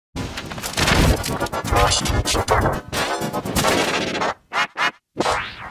Kaspersky Splaat
The next MTK Splaat will be Kaspersky Splaat, which is the 1st effect consisting of 2 colors.

The audio FX is changed because it was a counterfeit of both the old G-Major 13 and R-Major 2002. The video FX is changed for the better.

*Originally uploaded on 2 January 2022.*